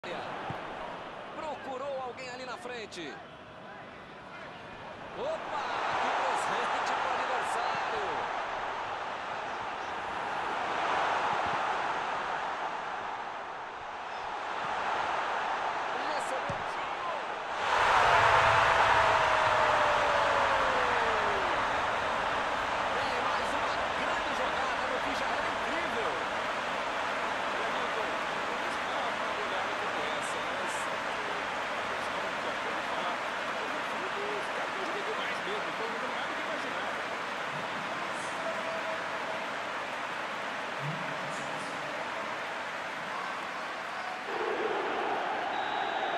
Recovery of a FAILED attack (Timothy Weah assists Martin Ødegaard!)
A failure to finish ultimately leads to redemption with a Timothy Weah feed to Martin Ødegaard. Ya love to see it!